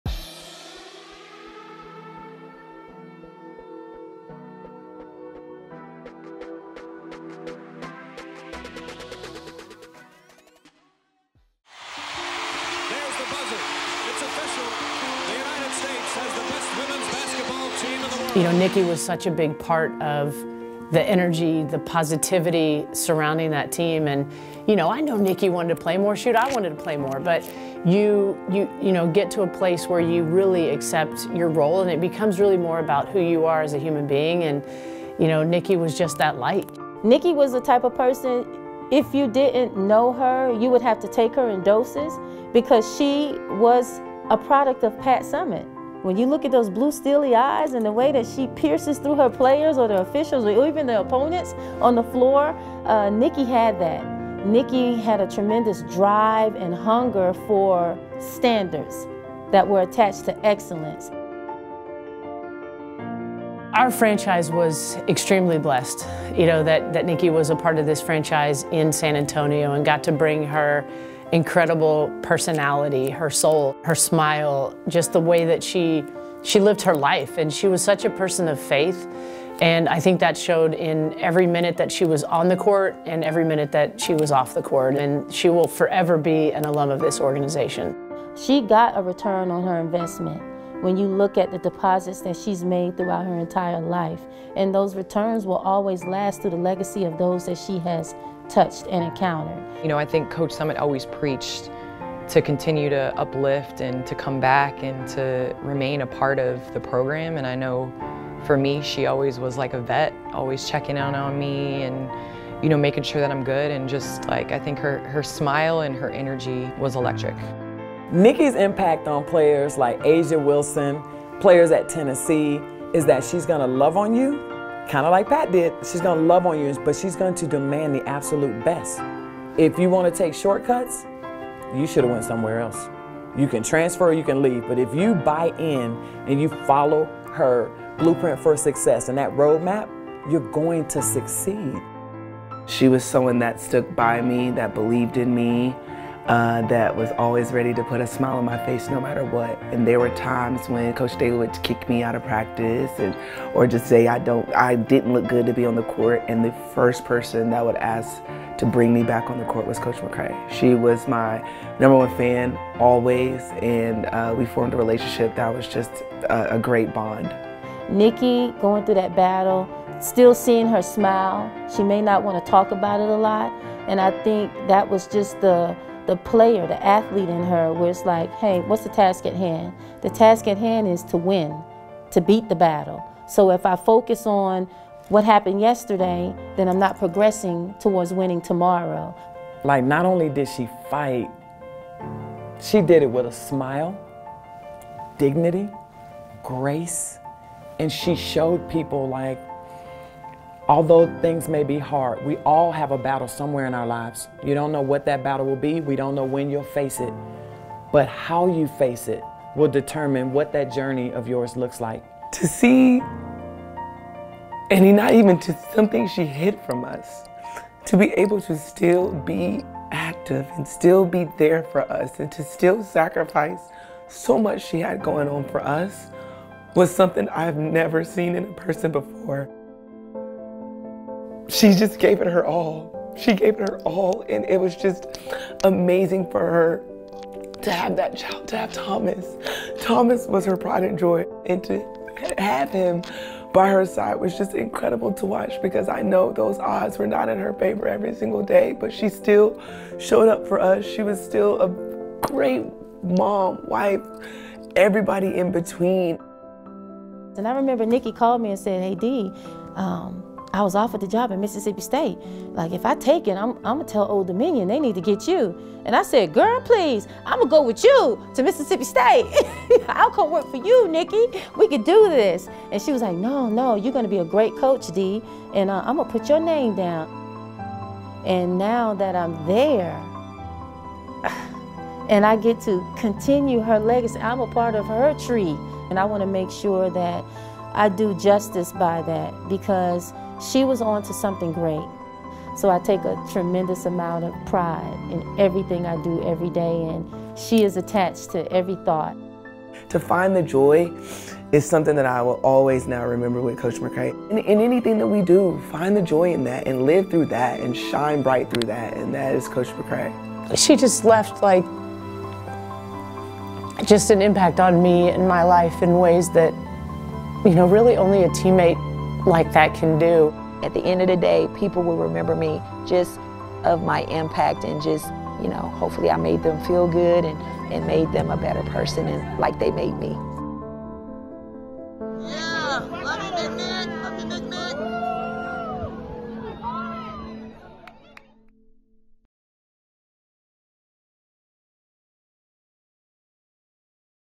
In The Paint With the Las Vegas Aces | A Las Vegas Aces Tribute to Nikki McCray-Penson
"To find the joy is something that I will always now remember with Coach McCray. In anything that we do find the joy in that. And live through that. And shine bright through that. And that is Coach McCray." — A'ja Wilson on Coach Nikki McCray-Penson